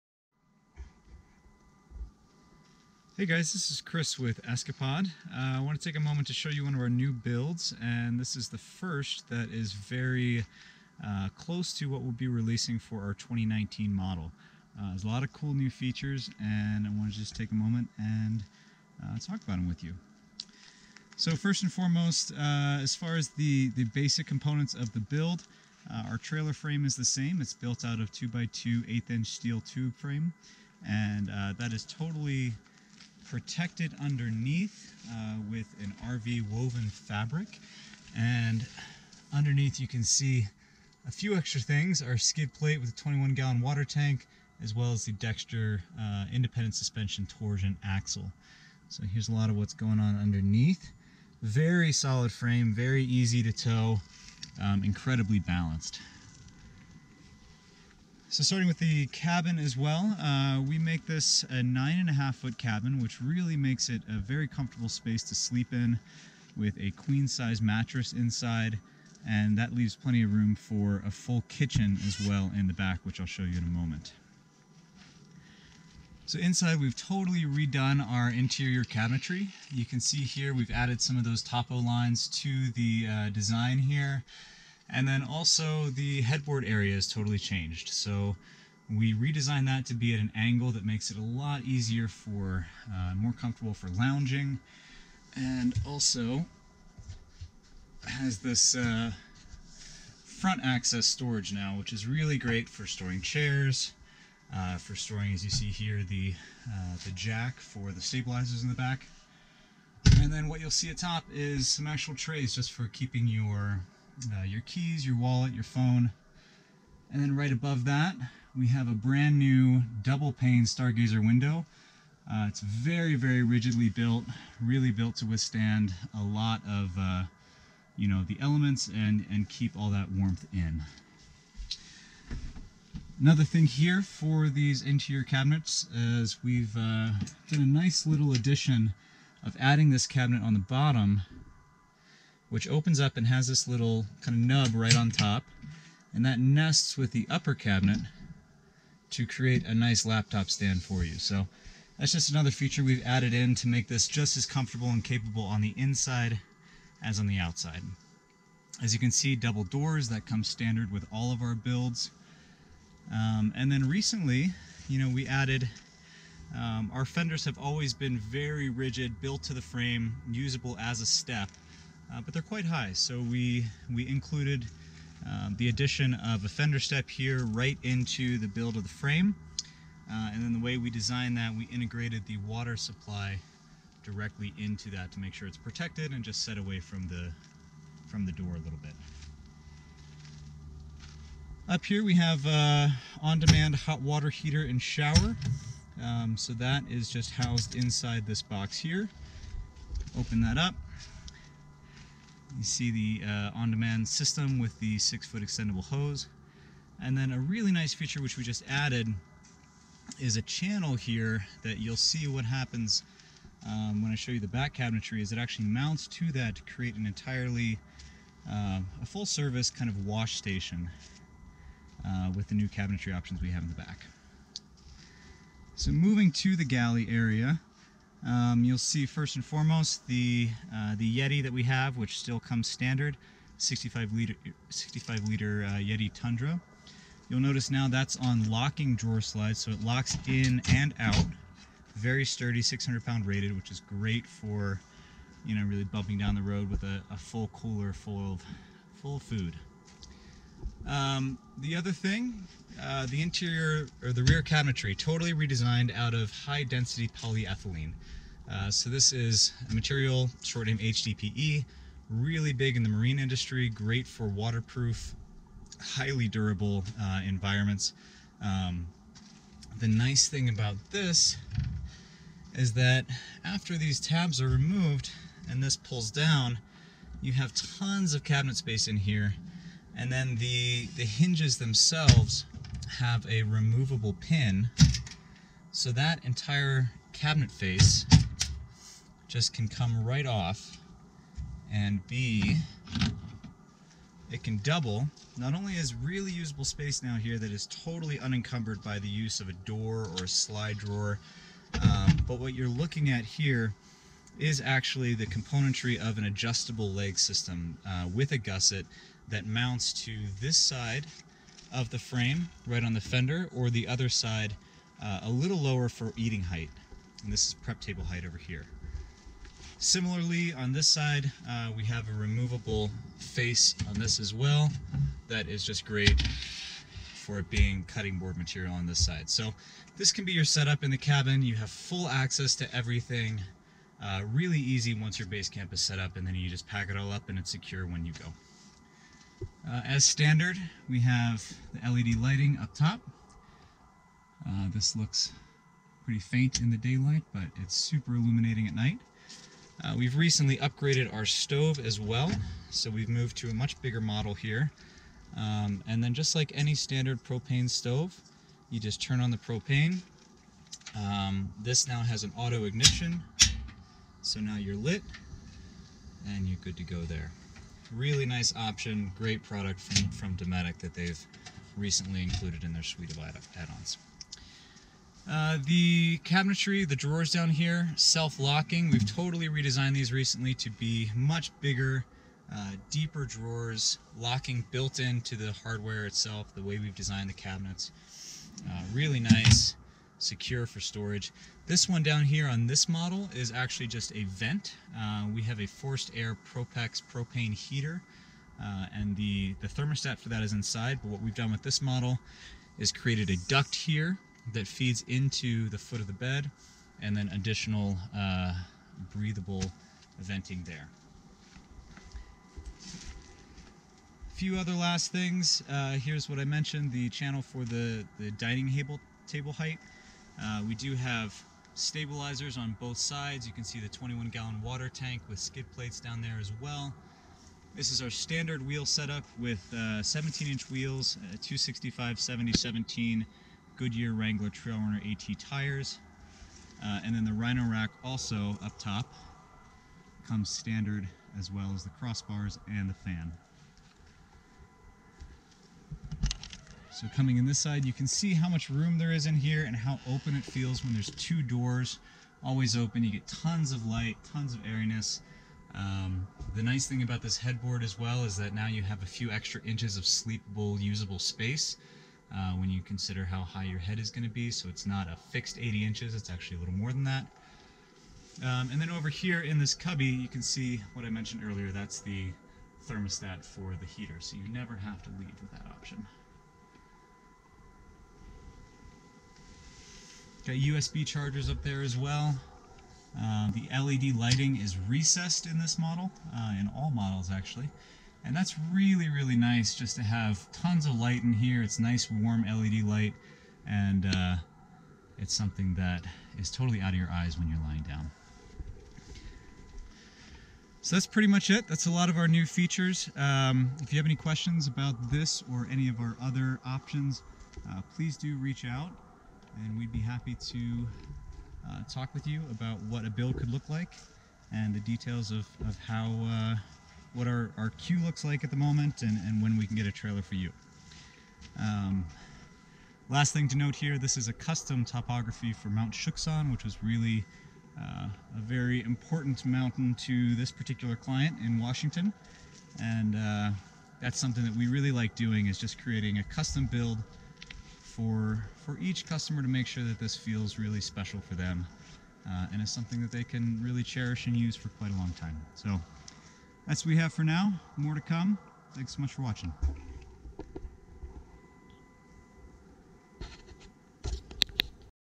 Escapod 2019 Teardrop Trailer Prototype
Model $ start at $16,500, as shown is $19,200. Solar, water, heater, AC, how water heater, rooftop tent - all are options. See full list of options and colors